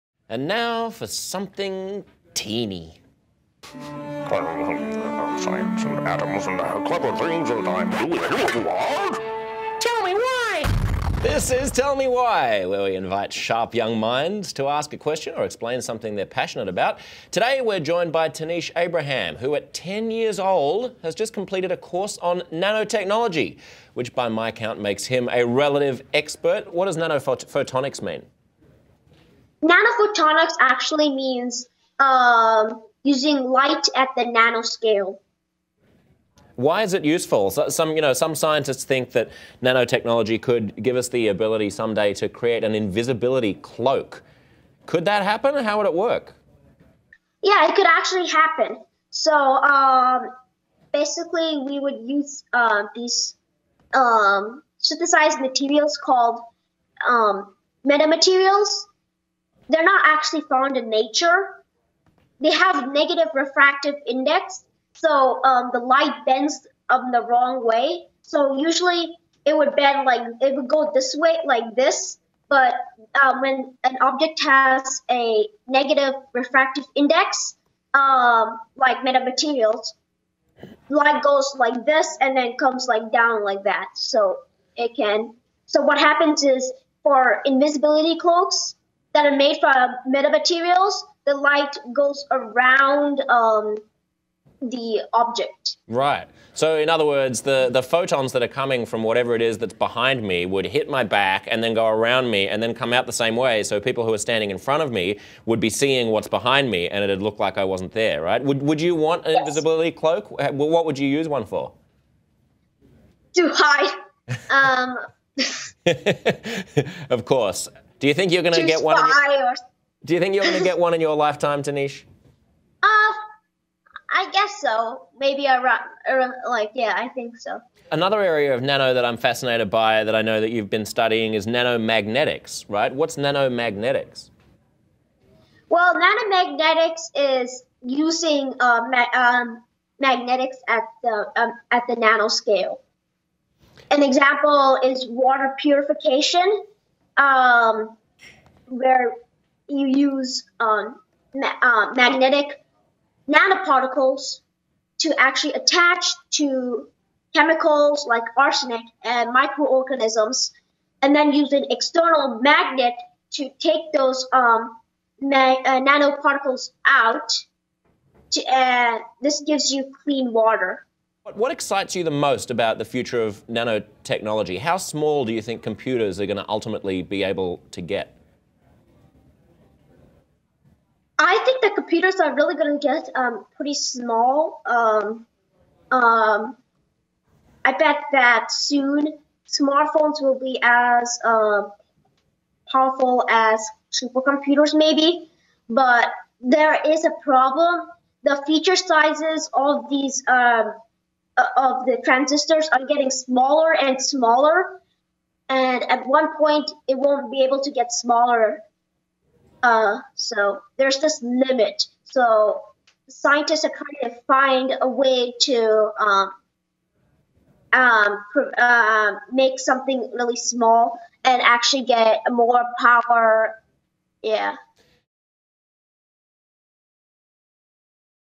What Is Nanotechnology?
Experts may have all the answers, but let's face it: kids ask the best questions. Unless of course they're 10-year-old Tanishq Abraham, who just completed a course on nanotechnology. Today, Tanishq explains what it is and how it works.